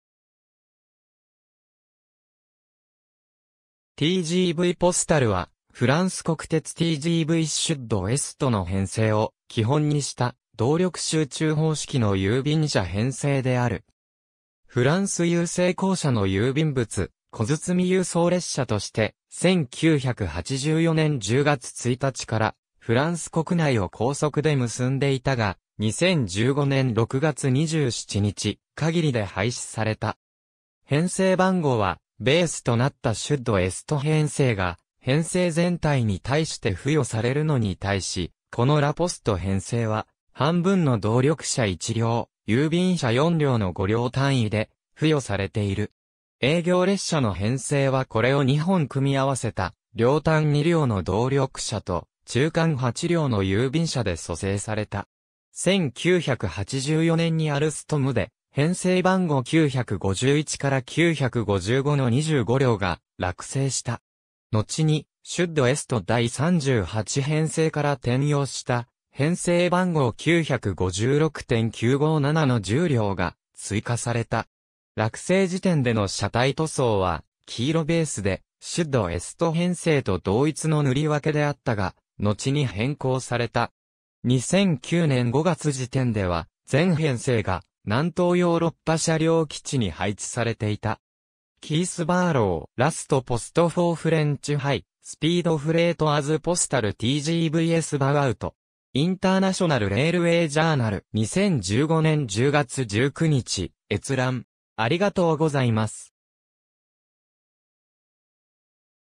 TGV La Poste
TGV postal (TGV La Poste) はフランス国鉄 (SNCF) TGV Sud-Estの編成を基本にした動力集中方式の郵便車編成である。
フランス郵政公社 (La Poste) の郵便物・小包輸送列車として、1984年10月1日からフランス国内を高速で結んでいたが、2015年6月27日限りで廃止された。
編成番号はベースとなったSud-Est編成が編成全体に対して付与されるのに対し、このLa Poste編成は半分の動力車1両+郵便車4両の5両単位で付与されている。
営業列車の編成はこれを2本組み合わせた両端2両の動力車と中間8両の郵便車で組成された。
1984年にアルストムで編成番号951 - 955（923000形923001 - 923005）の25両が落成した。
のちにSud-Est第38編成から転用した編成番号956・957（923006・923007）の10両が追加された。
落成時点での車体塗装は黄色ベースで、Sud-Est編成と同一の塗り分け（「LA POSTE」のロゴが車体のダークグレイの帯の中に入る）であったが、のちに変更された（写真参照）。
2009年5月時点では全編成が南東ヨーロッパ車両基地 (Technicentre Sud Est Européen) に配置されていた。
^ Keith Barrow (2015年6月29日). “Last post for French high-speed freight as postal TGVs bow out”. International Railway Journal. 2015年10月19日閲覧。